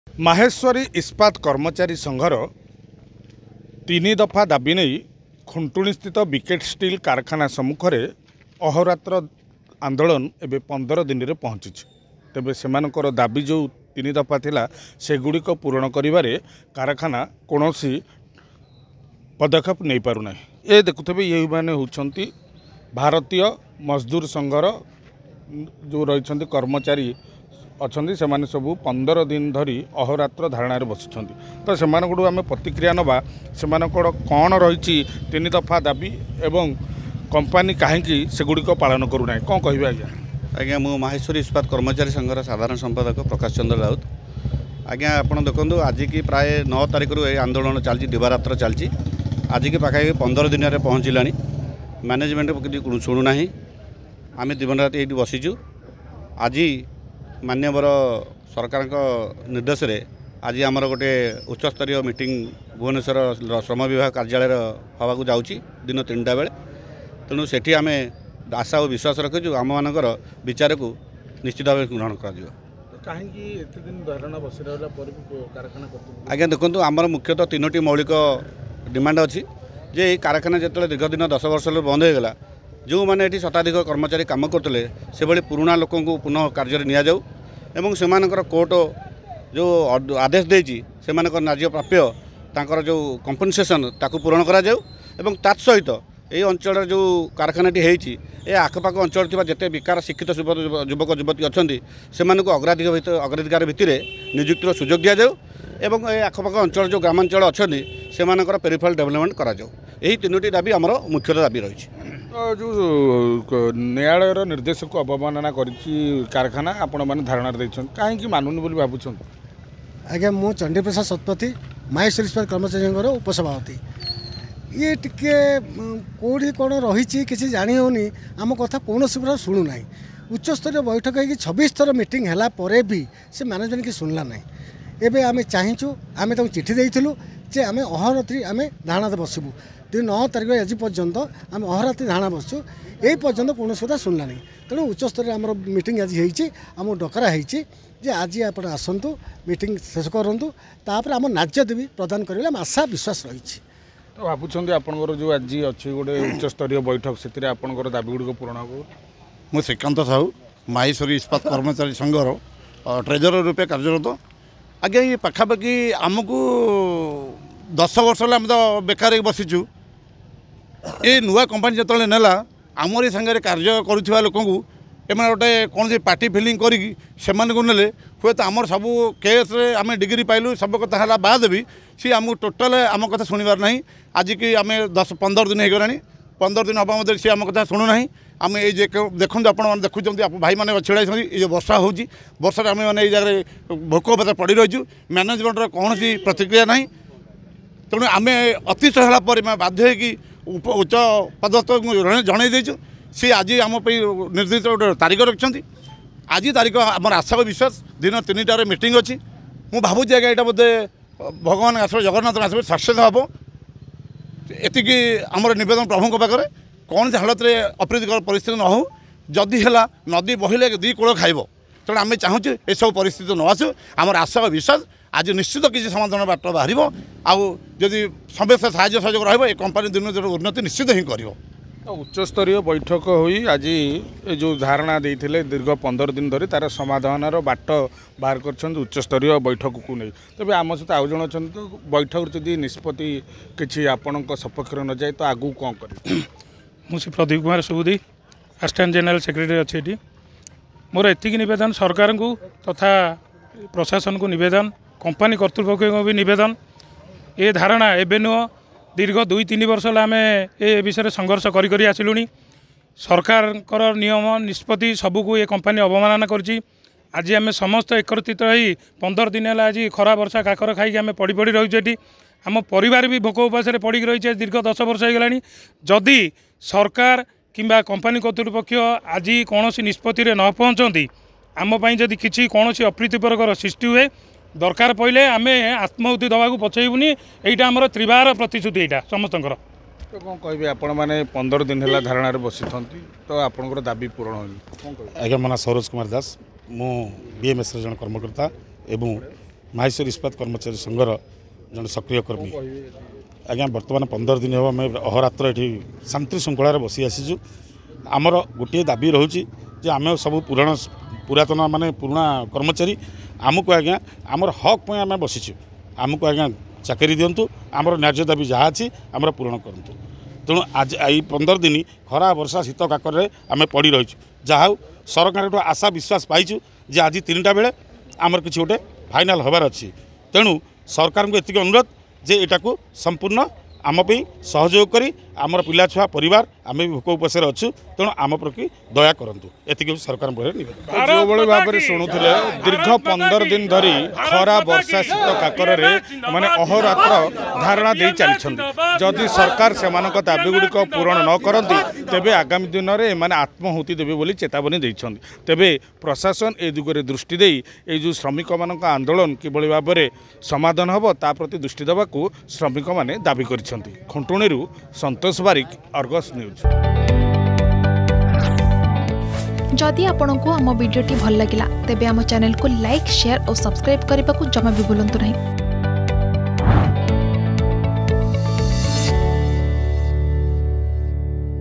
କାର୍ଯ୍ୟବନ୍ଦ ଆନ୍ଦୋଳନରେ ବିକେ ଷ୍ଟିଲର ଶ୍ରମିକ || Labour Protest || Athagarh
ମାହେଶ୍ୱରୀ ଇସ୍ପାତ କମ୍ପାନୀରେ ପୂର୍ବରୁ କାର୍ଯ୍ୟରତ ଶ୍ରମିକମାନଙ୍କୁ ପୁନର୍ବାର ନିଯୁକ୍ତି ଦେବା ସହିତ ସେମାନଙ୍କର ବକେୟା ବେତନ ପ୍ରଦାନ କରାଯିବା ପାଇଁ ଚୁକ୍ତି ସ୍ବାକ୍ଷର କରାଯାଇଥିଲା। ହେଲେ ଦୀର୍ଘ ମାସ ପରେ ବି କର୍ମଚାରୀ ମାନଙ୍କୁ ନିଯୁକ୍ତି ଦିଆଗଲା ନାହିଁ । ଅନିର୍ଦ୍ଦିଷ୍ଟ କାର୍ଯ୍ୟବନ୍ଦ ଡାକରା ଦେଇଥିବା ବେଳେ ଆଜି ୧୫ ଦିନରେ ପହଂଚିଛି ଆନ୍ଦୋଳନ ।